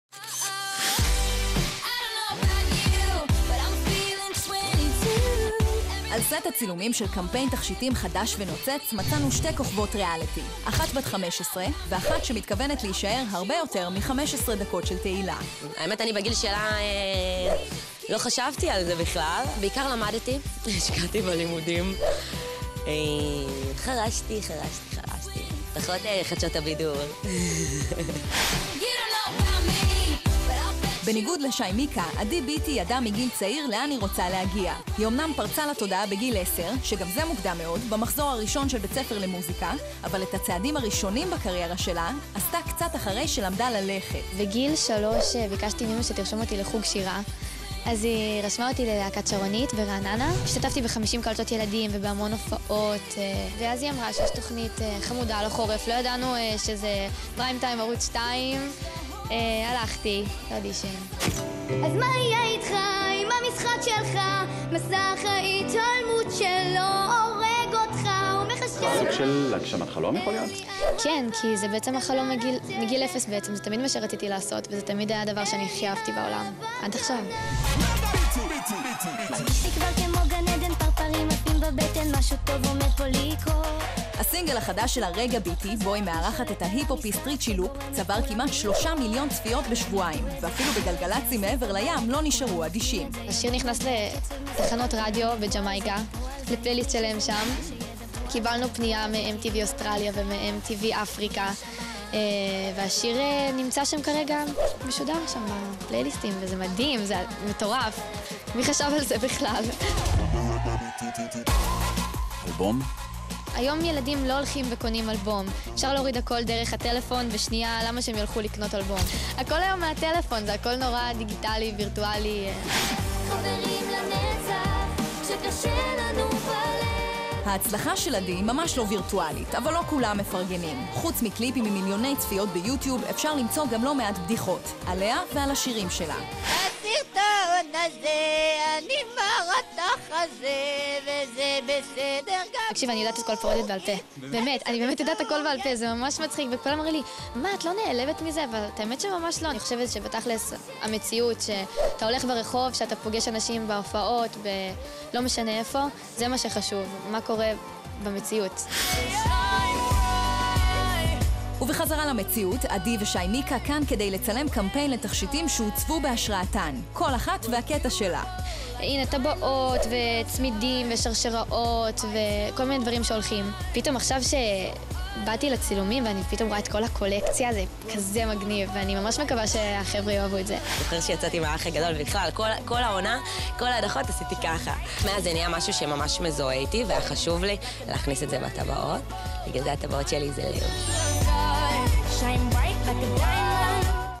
עדי ביטי ושי מיקה מצטלמות - חדשות הבידור
מי הזמרת הישראלית בת החמש עשרה שכובשת את תחנות הרדיו בעולם ומה הקשר לשי מיקה?